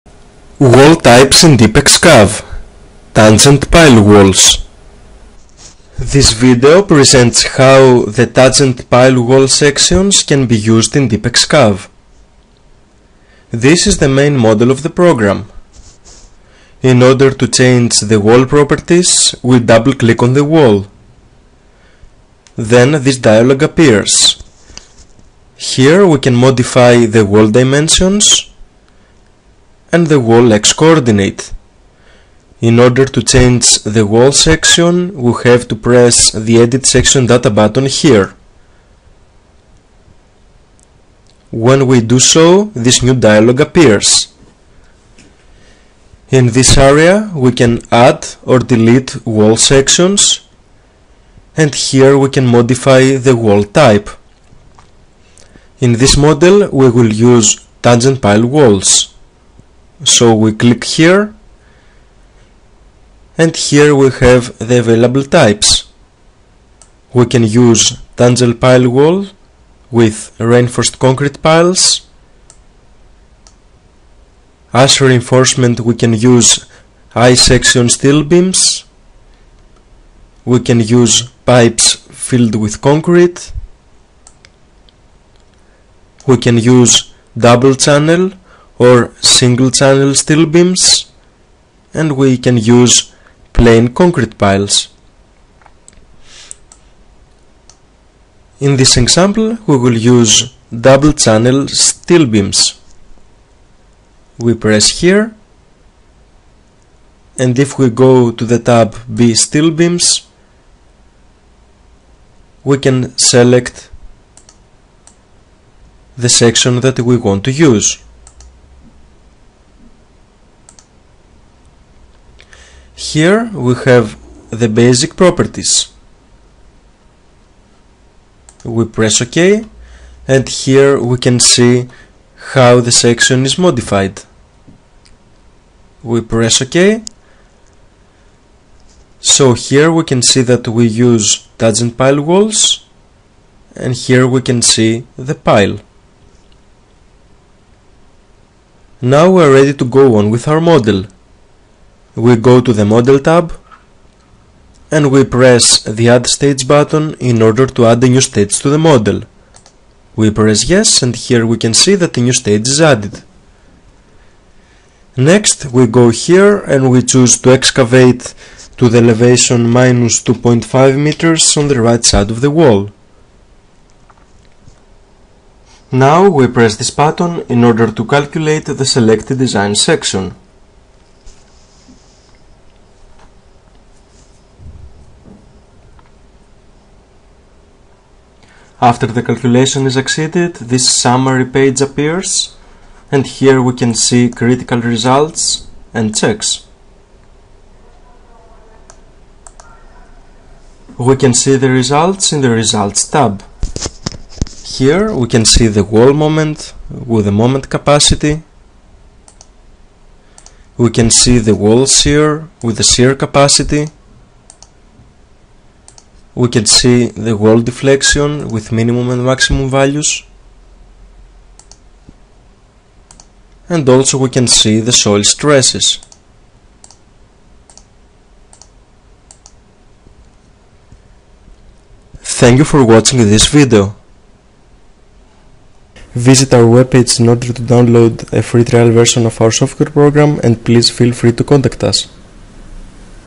Tangent Pile Walls
In this short video, we show you the secant ta sections we can create by the tools of DeepXcav - best geo engineering software for geo technical engineers, civil engineers. Feel free to arrange a free live online demonstration with our leading geo technical experts.